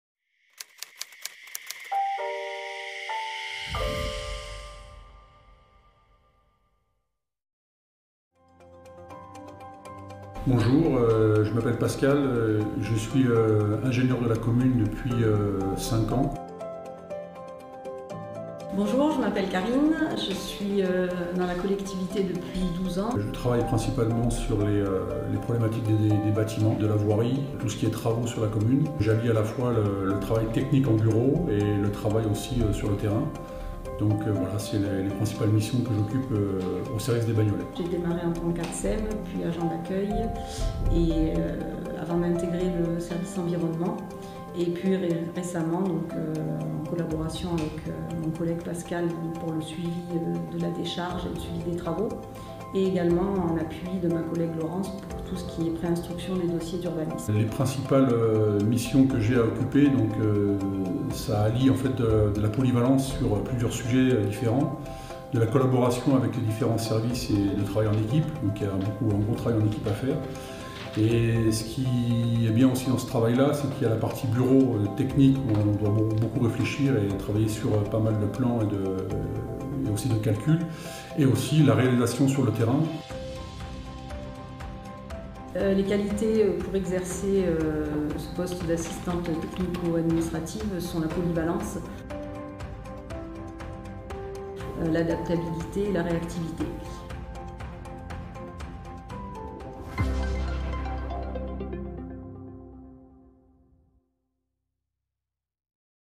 Service Travaux et Environnement
Ce binôme est chargé de concilier ce qui n'est pas toujours conciliable, le développement et les travaux pour le compte de la commune et la préservation de l'environnement. Ils œuvrent en parallèle pour la Société Publique Locale du Vallon des Pins pour préparer la sécurité et la transparence de fonctionnement du futur site de la décharge.